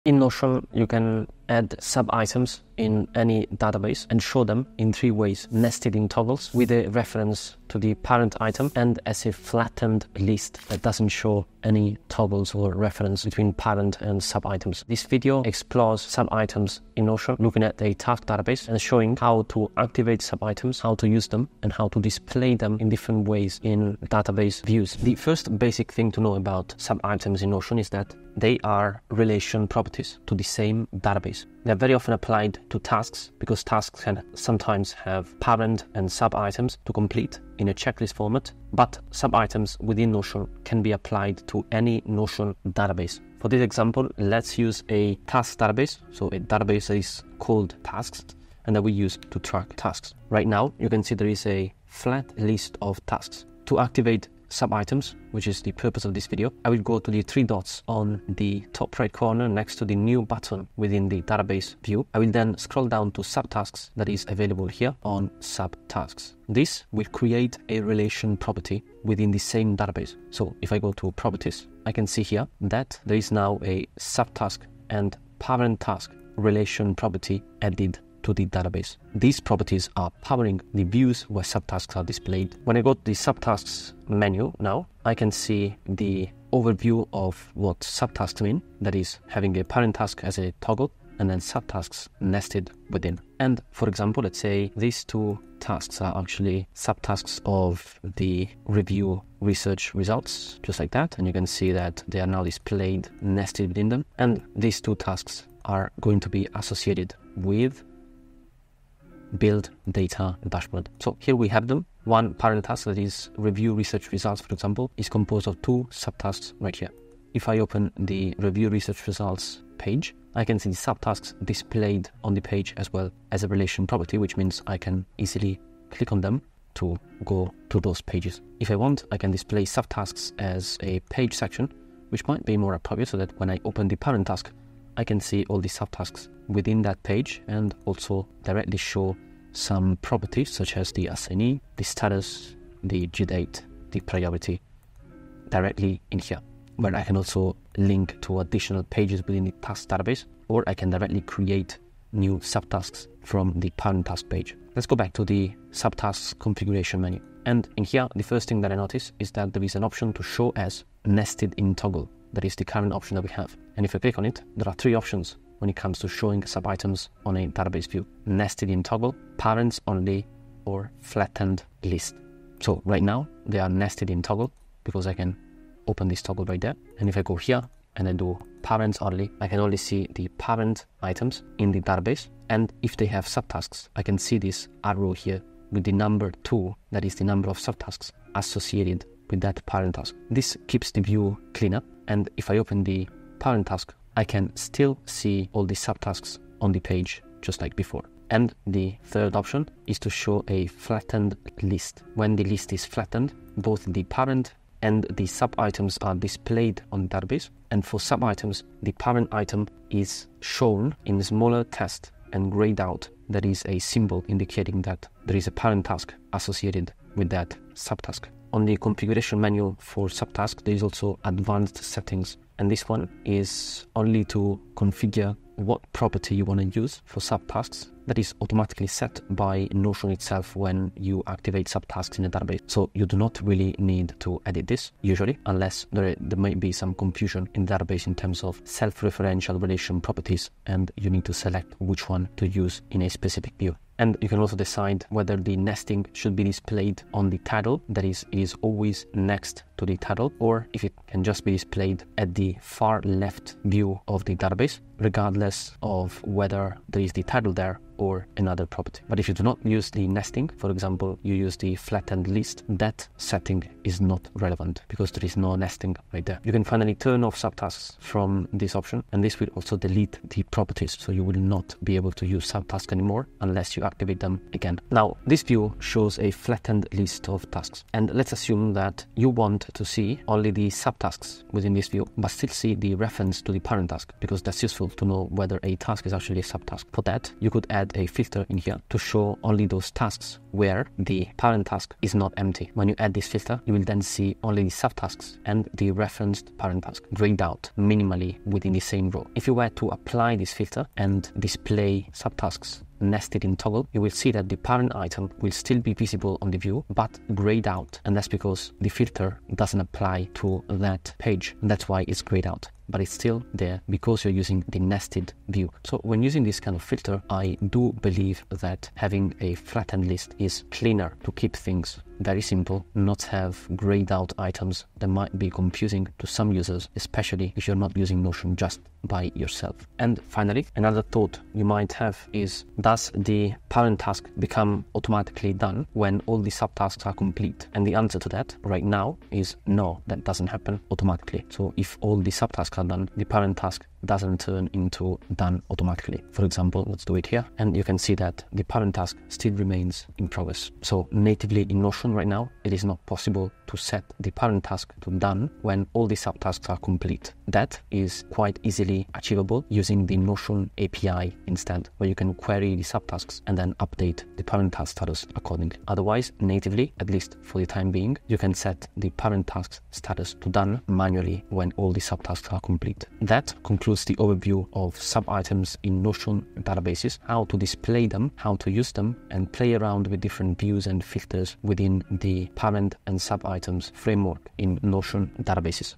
How to use Notion sub-items (sub-tasks)
- Build your web forms with Tally (integrates natively with many tools)

TIMESTAMPS
0:00 The three ways to show sub-items in Notion
0:25 How to activate sub-items
2:00 How to display sub-items on the parent page
2:35 How to show sub-items in database views
4:35 Filtering sub-items in database views
5:30 Automatic parent task status?
6:12 Conclusion